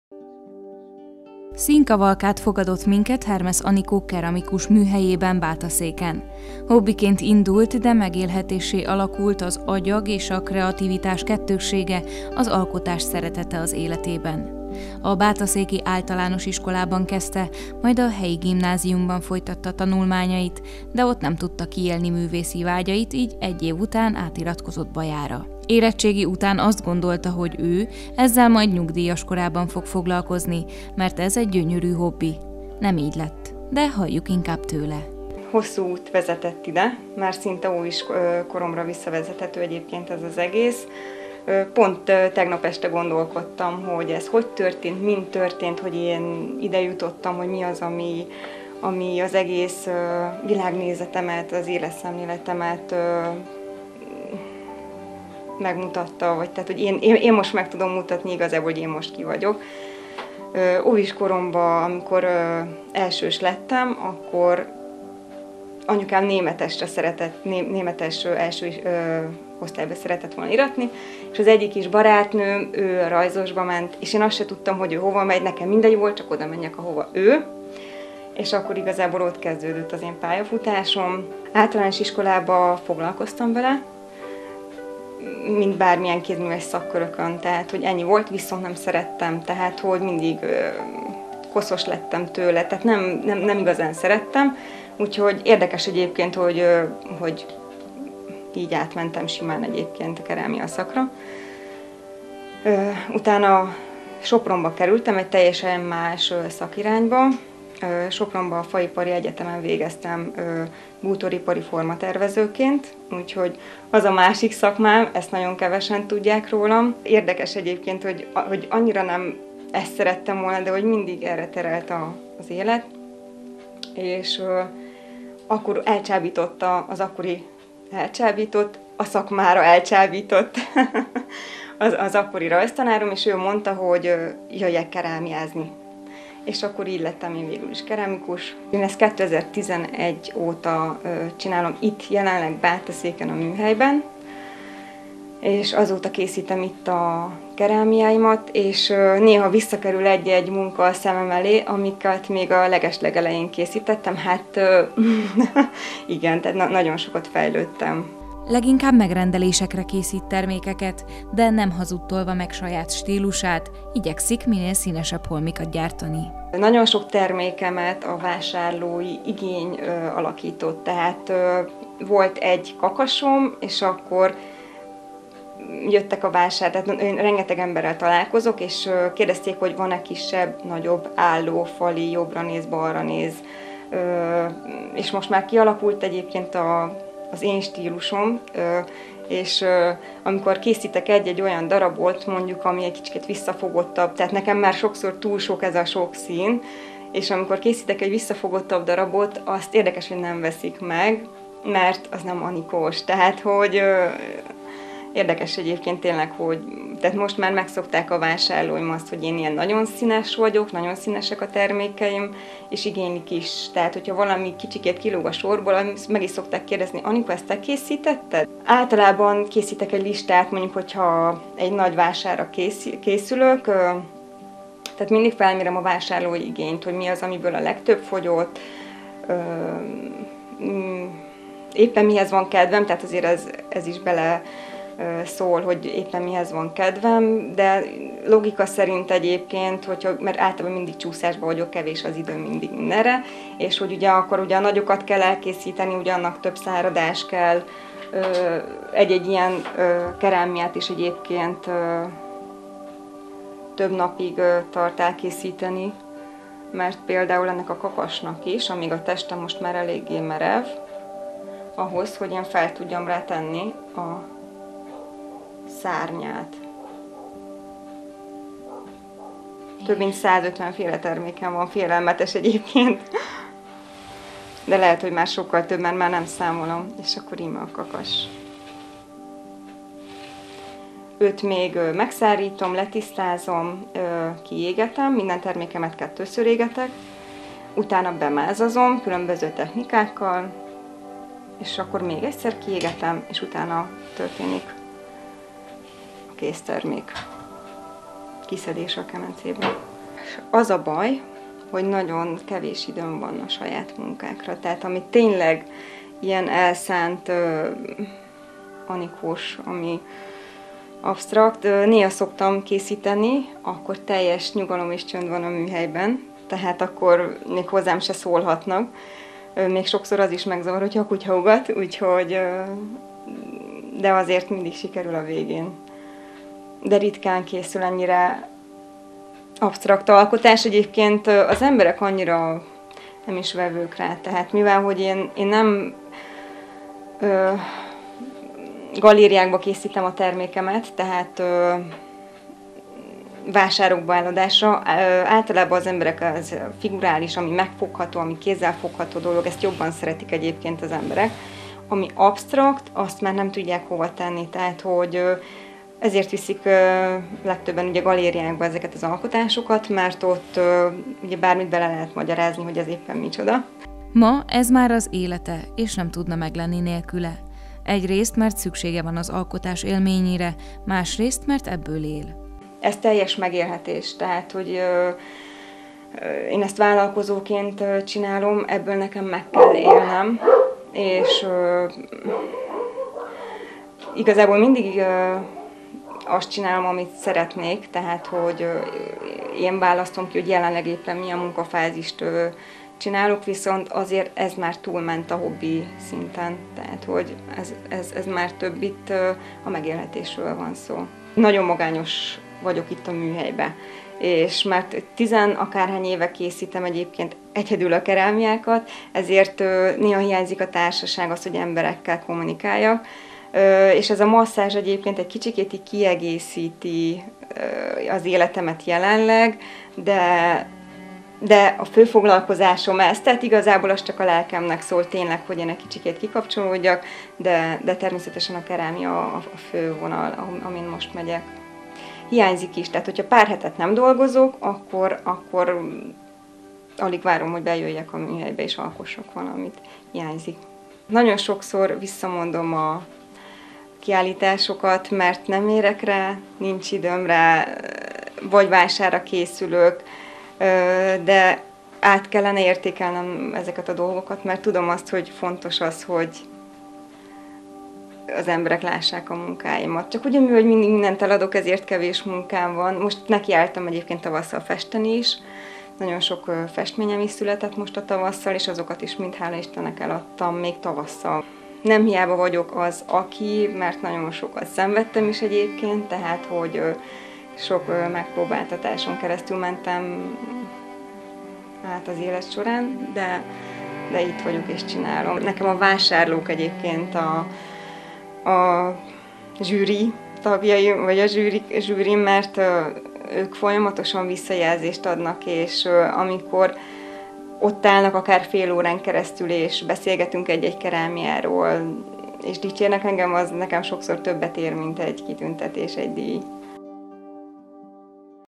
Hermesz Anikó keramikus - Bátaszék 20200720
Gyermekkora óta része az életének és 2011-től van saját műhelye is. Nagyon színes stílusa miatt már sokan megismerik a munkáiát. Hermesz Anikó keramikussal beszélgettünk.